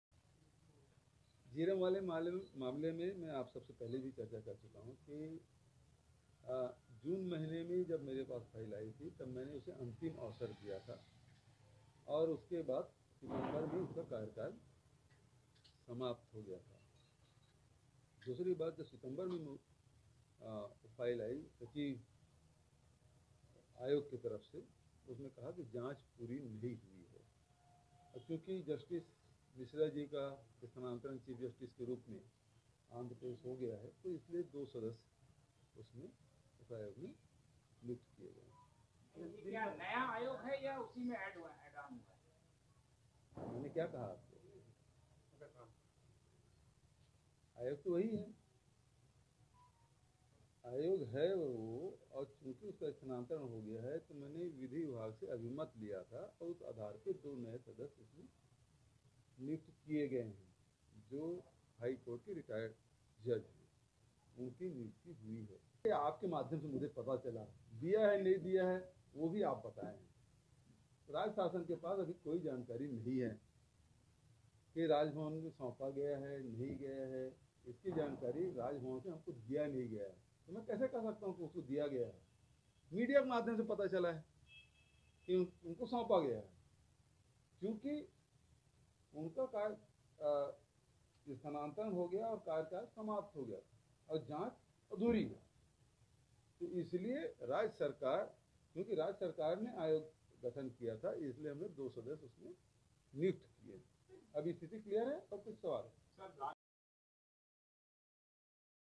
Jhiram Valley Attack: जांच आयोग का कार्यकाल 6 माह के लिए बढ़ा | Jhiram Valley case Chhattisgarh
बहुचर्चित झीरमघाटी हत्याकांड (Jhiram Ghati massacre) की जांच में सरकार ने आयोग के कार्यकाल में 6 महीने की वृद्धि कर दी है. इसके साथ ही सरकार ने इस आयोग में दो नए सदस्यों की नियुक्ति की है.